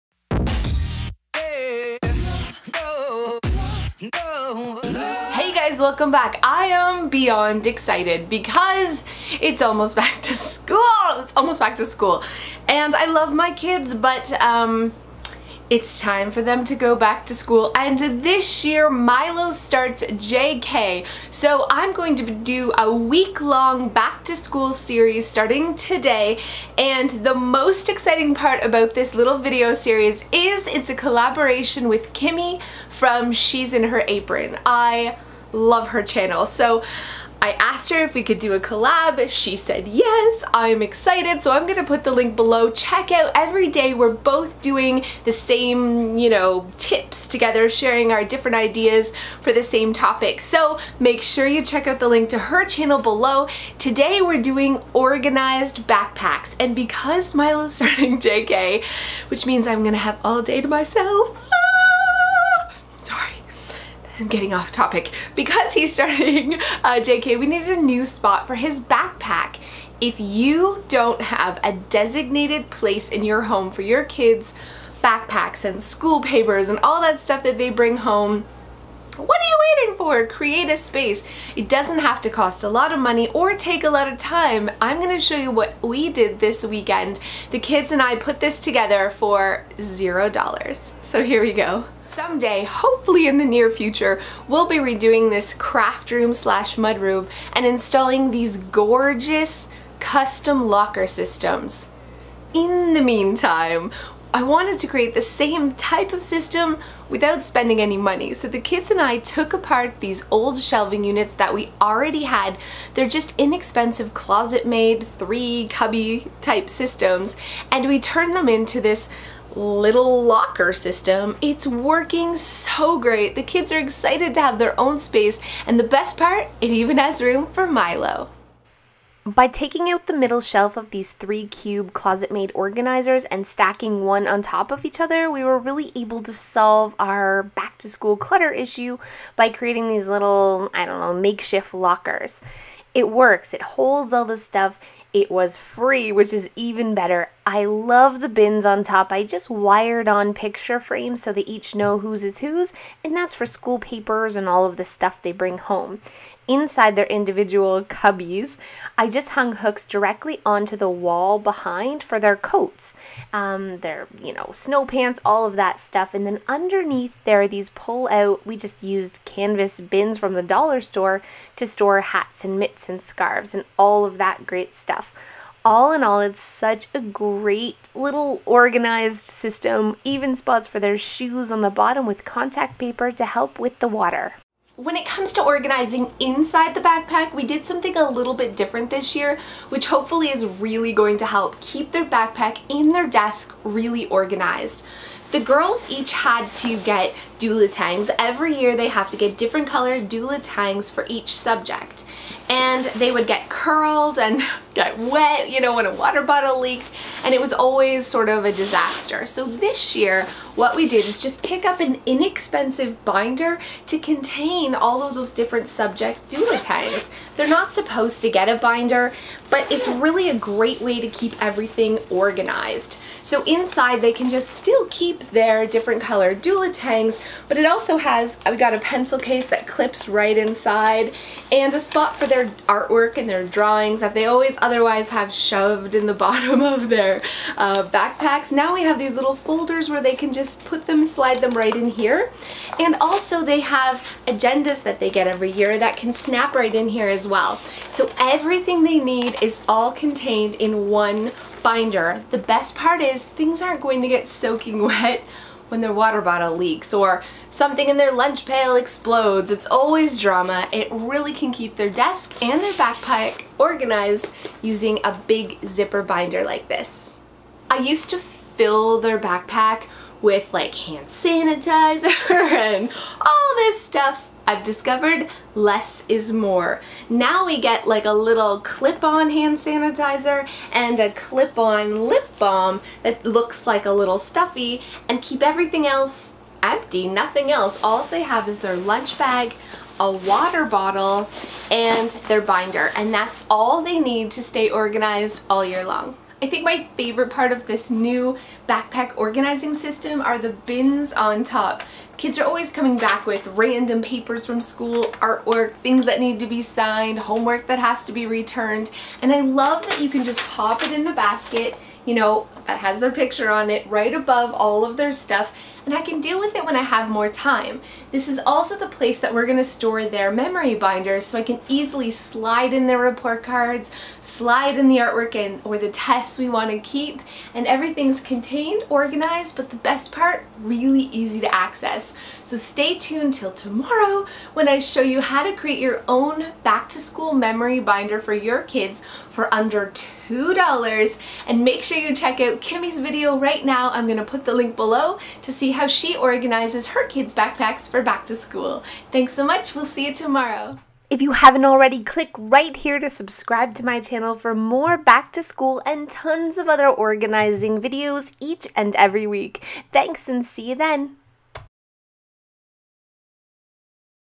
Back to School Backpack Organizing Tips
Let's get organized!
Back to School is almost here! Here are some backpack organizing tips that really work for our kids!  We created a backpack station and I have a few tips to keep your backpack and desk clean and clutter free!

Anyone else excited for Back to School 2016??

Pre-order my NEW book for just $11.57